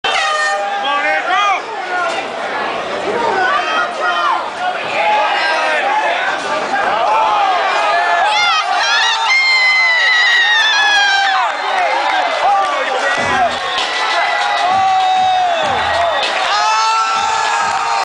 Allen Homan Knocks out Andrew Espling @ Club Abyss
Allen Homan Knocks Out Andrew Espling @ Club Abyss in Sayersville, NJ